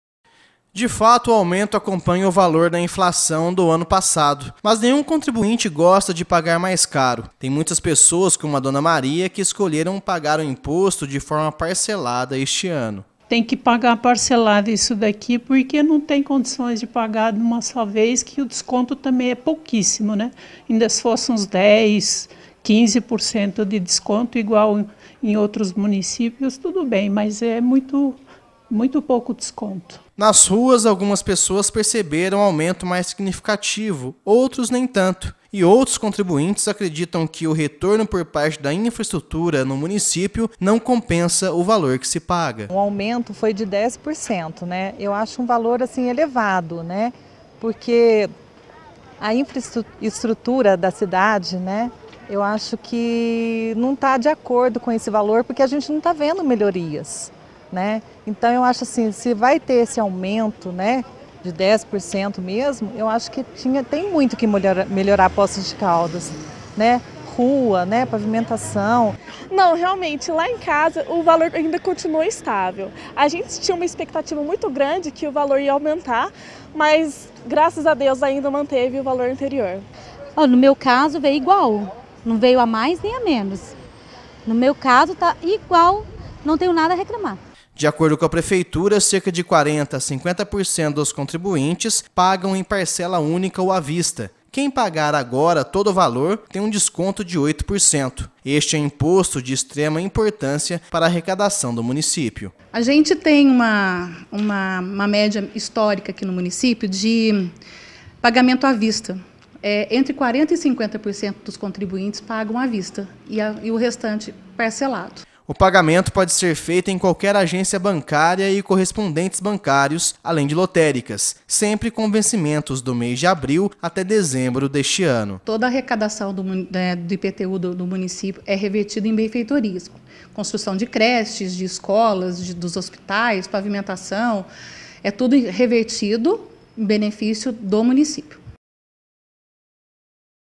INÍCIO PAGAMENTO IPTU
Contribuintes já estão pagando os carnês do IPTU, uns à vista e outros parcelados.